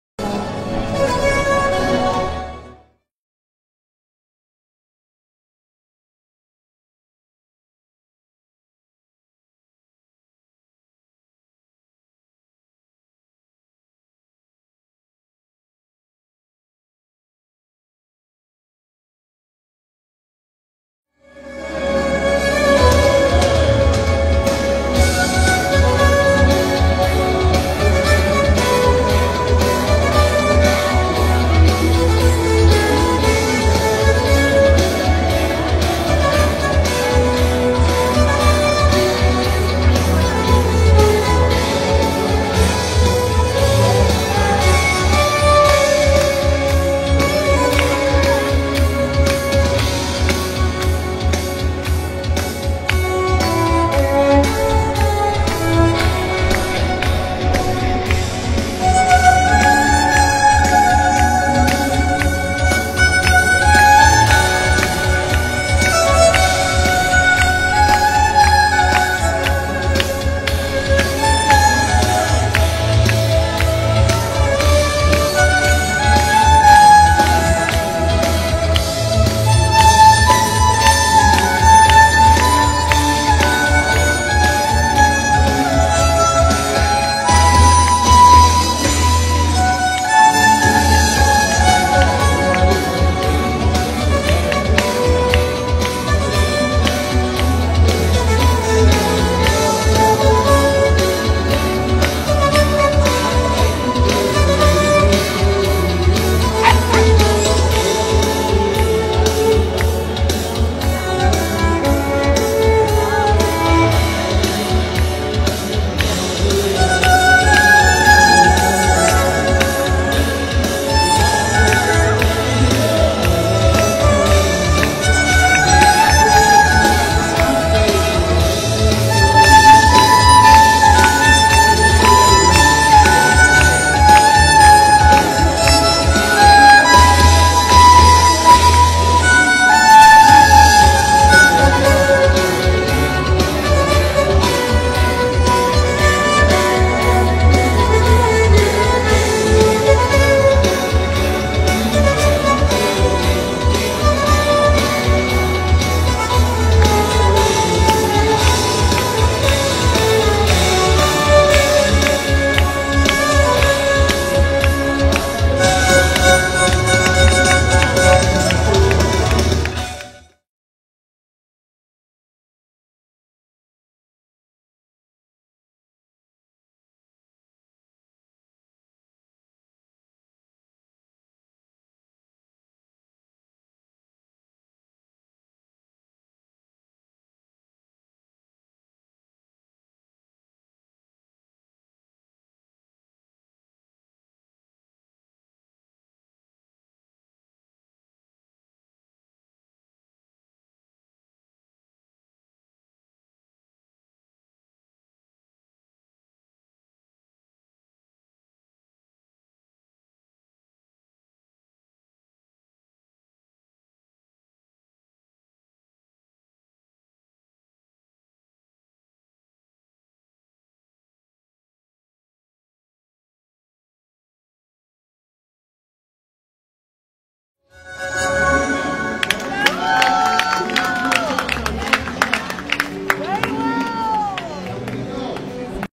Amazing Violin Performer.. Meet Karolina Protsenko
Video tentang seorang gadis kecil yang sangat berbakat bermain biola..!!